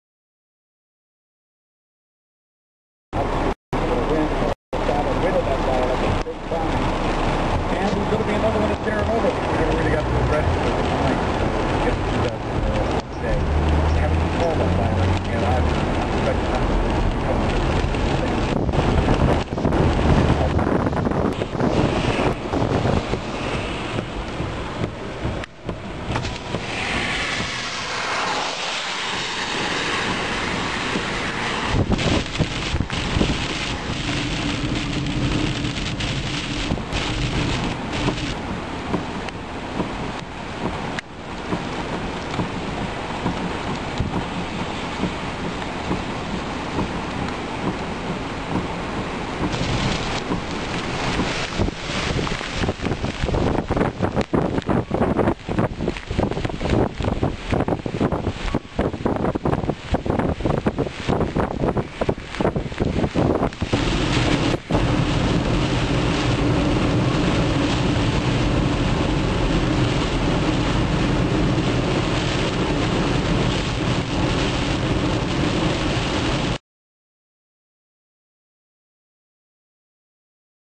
Fall Storm
Gale force winds and heavy rains and snow pummel the east coast of the province in an vicious fall storm.  Here are some images in and around the St. John's area.